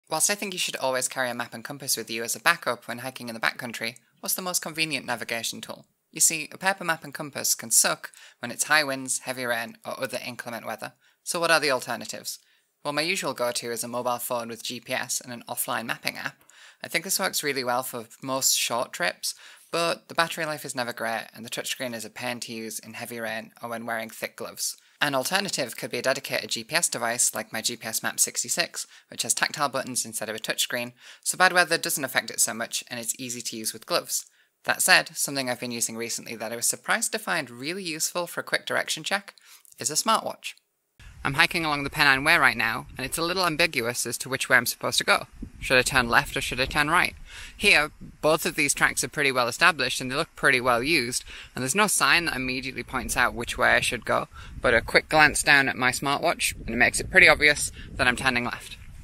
A Quick Navigation Check
Using a map and compass in inclement weather can suck; so what's the most convenient navigation tool for a quick direction check?

I was surprised to find a smart watch with mapping really convenient on a recent trip - especially when the weather really closed in!

This week I'm trying out the Garmin Epix 2